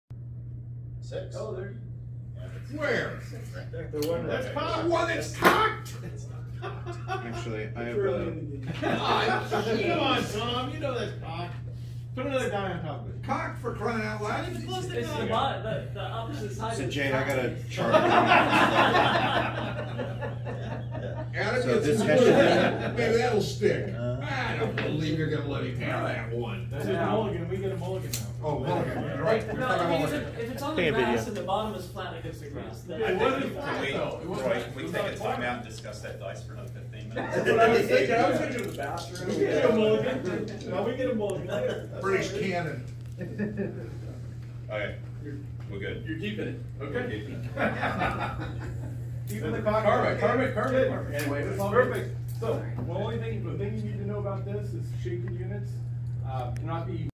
Guilford Courthouse Wargame (Sept 2021)
A short video I took of my AWI Guilford Courthouse wargame I hosted during CabinCon in September of 2021.